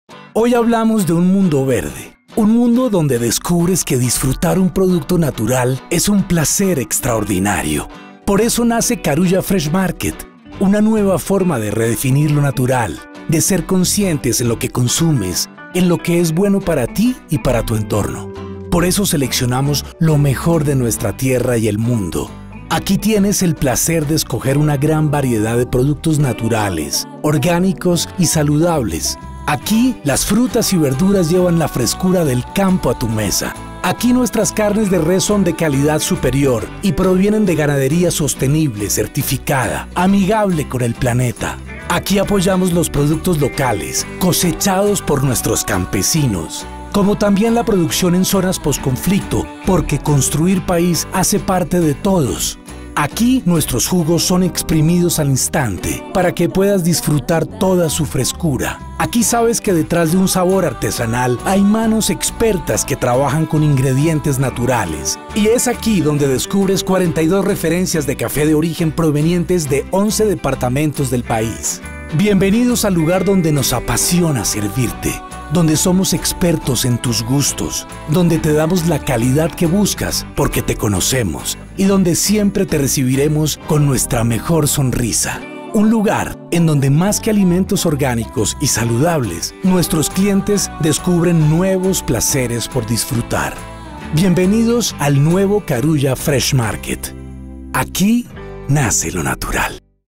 ¡Bienvenidos al nuevo Carulla FreshMarket!
En este novedoso formato nace lo natural, orgánico y sostenible. Te invitamos a que lo conozcas y vivas la frescura y calidad de nuestros productos.